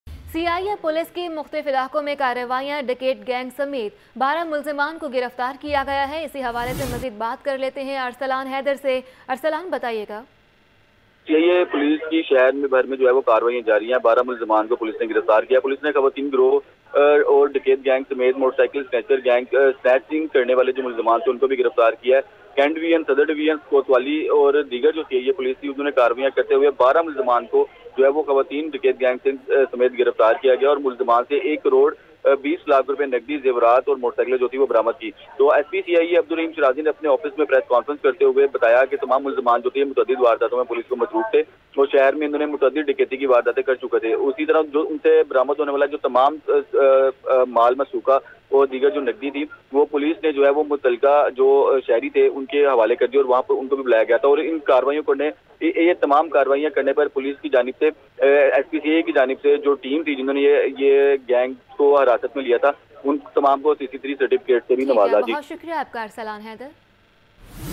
CIA Police apprehends 12 gangsters involved in various robberies
CIA Police apprehends 12 gangsters involved in several robberies